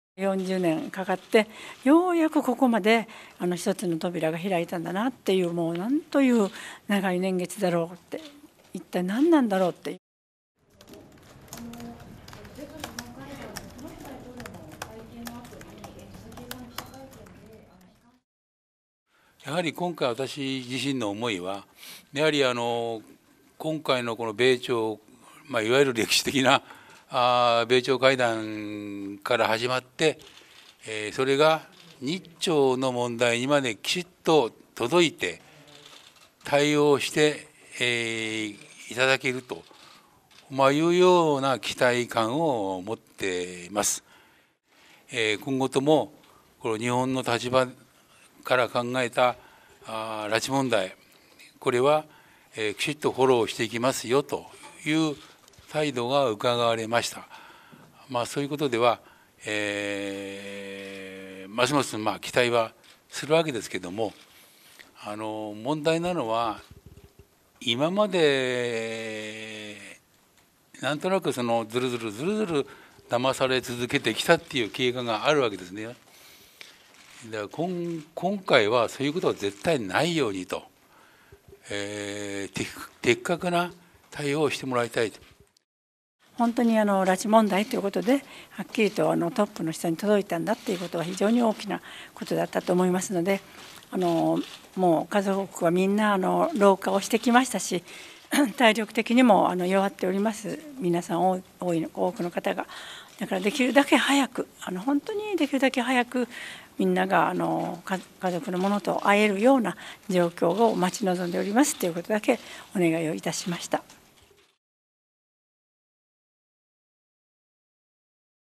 家族会「的確に対応を」 首相と面会後に記者会見
北朝鮮による拉致被害者家族会は14日、官邸での安倍晋三首相との面会後、国会内で記者会見を開いた。ますます期待している」と述べ、政府が9月開催を目指しているとされる日朝首脳会談に望みを託した。